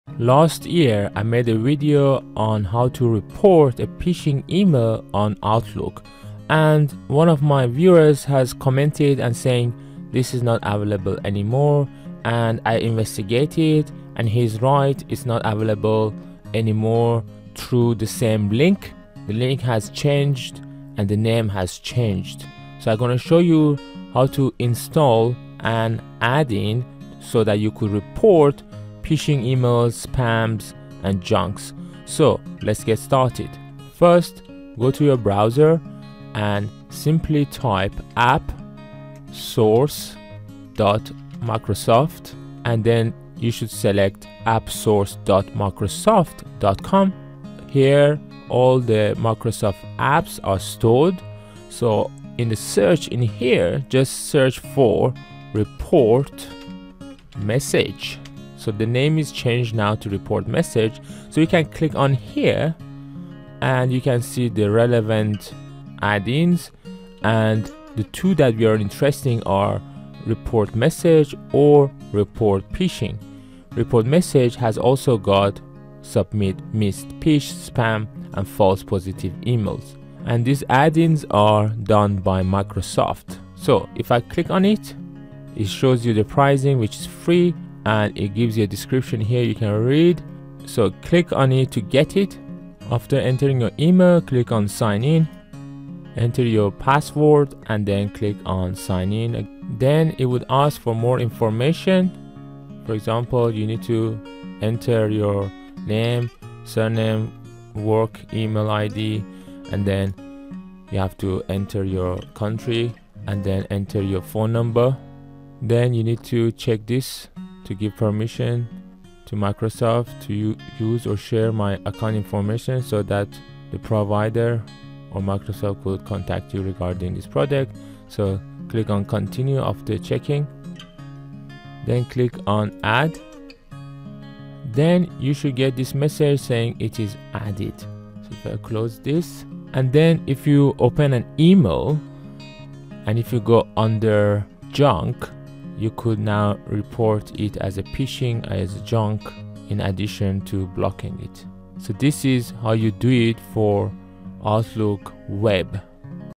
How to Report phishing in Outlook
In this quick tutorial on Outlook, I'll show you how to report phishing or spam. In my previous video, I've shown you how to do so by downloading an add-in for Outlook. As one of my viewers has pointed out, the link to that add-in is not available any longer. So, in this video, I'll show you another method that works for Outlook Web. As per the description, you could install it on Outlook 2016 and above. However, after a lots of tries, I wasn't able to install it my Outlook 2019. But, by following this method, you might be more successful than me. It might work if you've got Office 365. All the best!